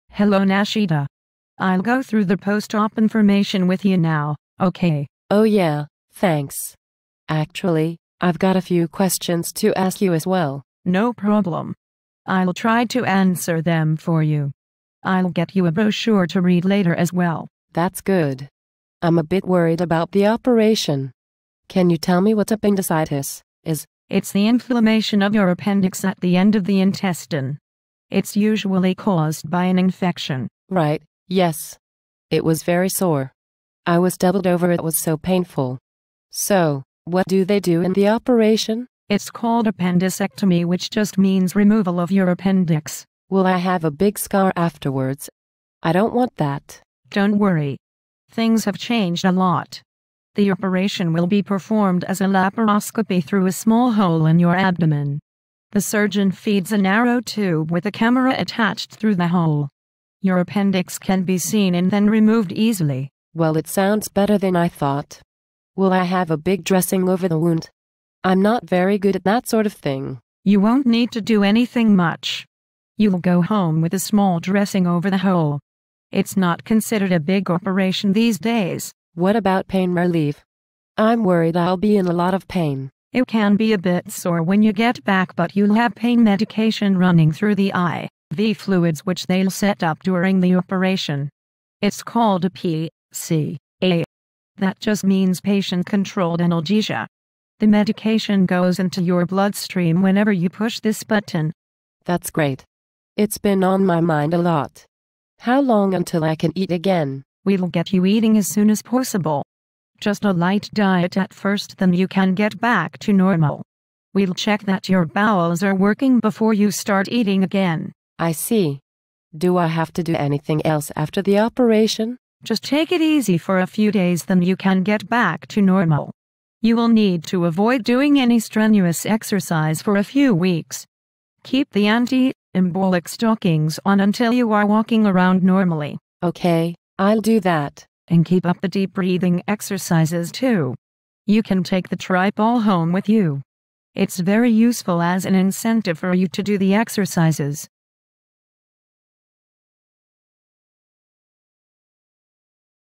English for Nurses: Explaining Post-operative Care of Appendicectomy
Learn how a nurse gives a patient information about the post-op care of appendicectomy.